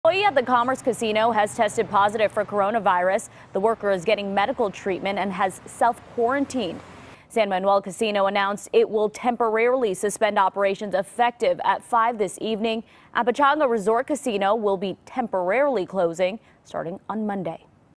Commerce Hotel & Casino Employee Presumed Positive For COVID-19
An employee of the Commerce Hotel & Casino has tested presumptive positive for COVID-19. Leslie Marin reports.